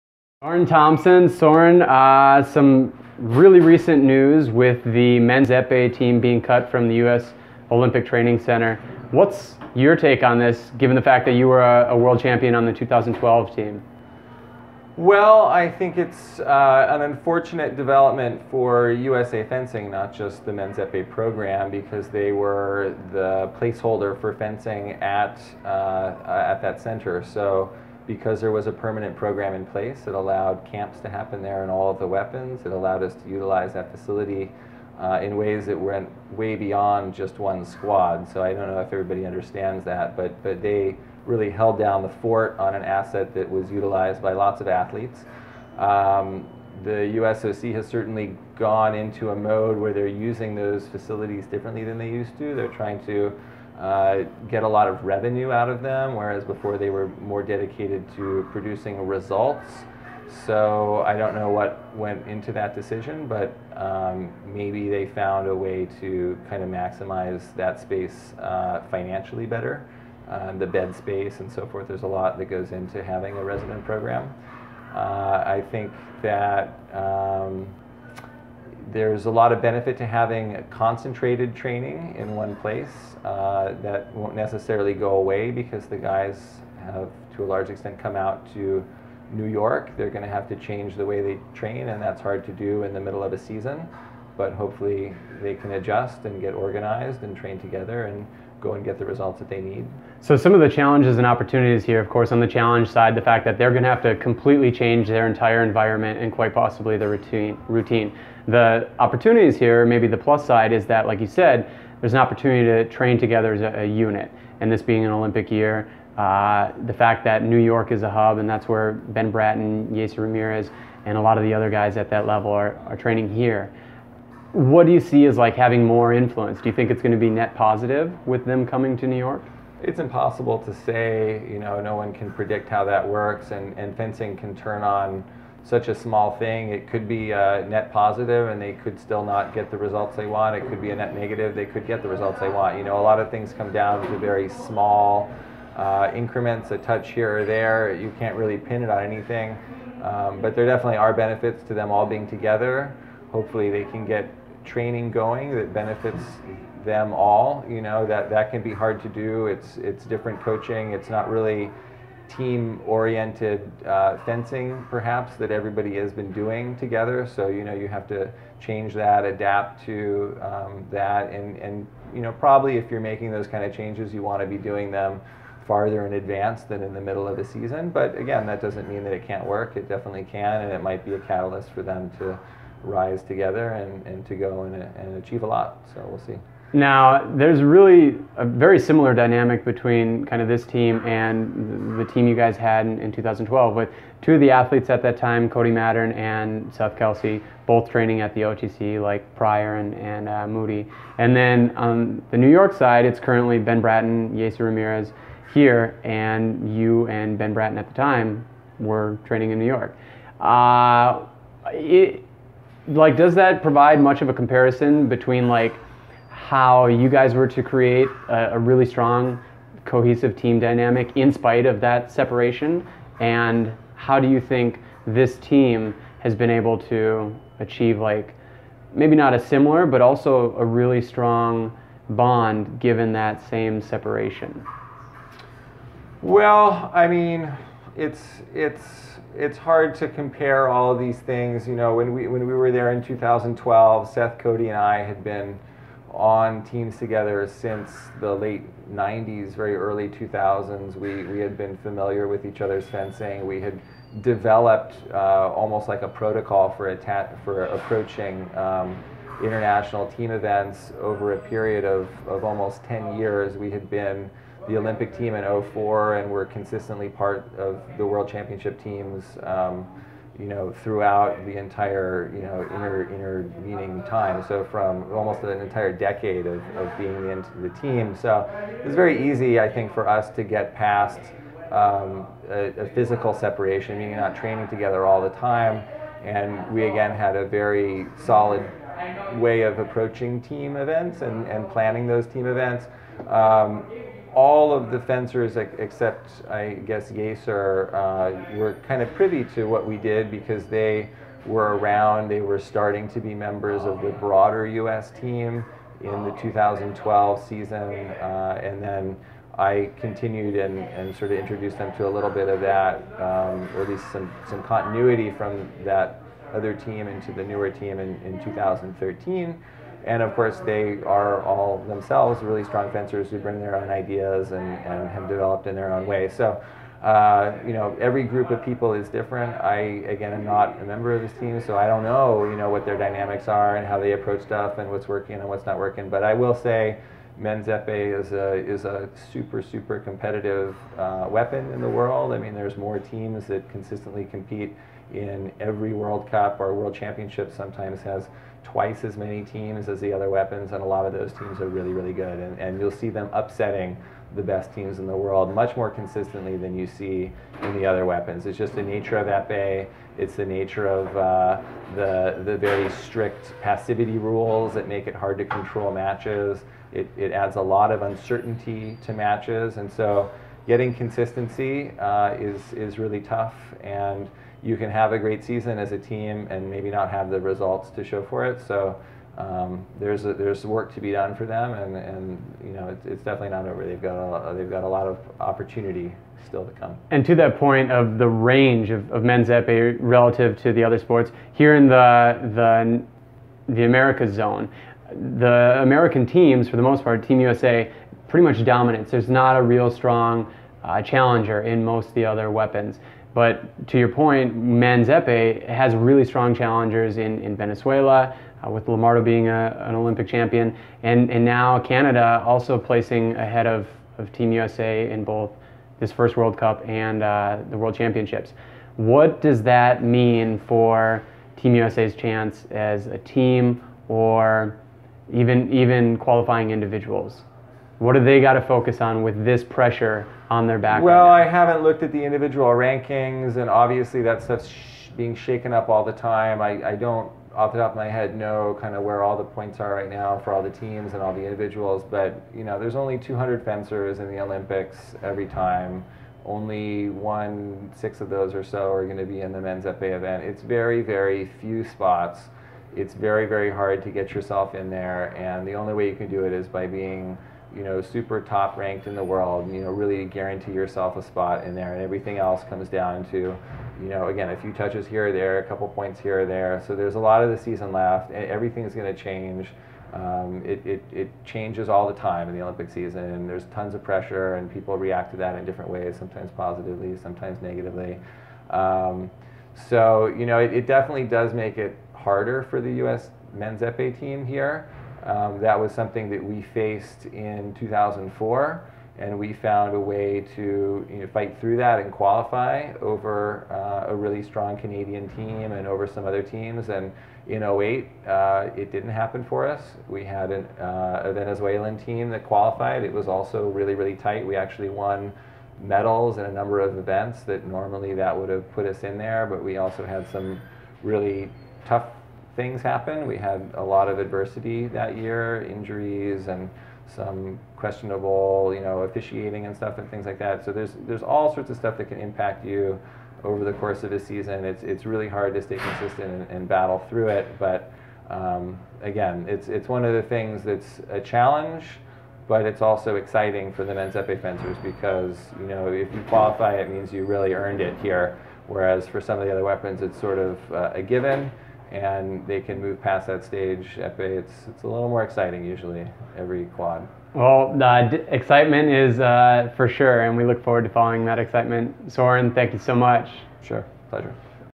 Soren Thompson's take on US Men's Epee
Two-time Olympian Soren Thompson talks about the OTC cutting Men's Epee, Team USA Men's Epee teammates, and qualifying within the Pan American zone.